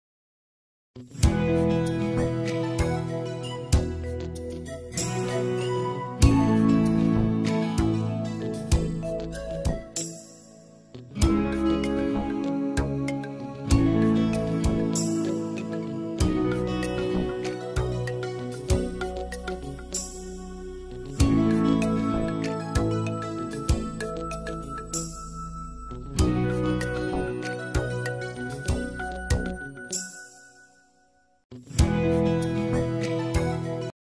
$7,500 in PILOT MTN 27041. Call Lou Jewell ALC
Undeveloped - Pilot Mountain, NC IN TOWN LOT NEXT TO VETERINARY OFFICE. FOR OVER 20,000 ACTIVE LSITINGS Find out more about this property for sale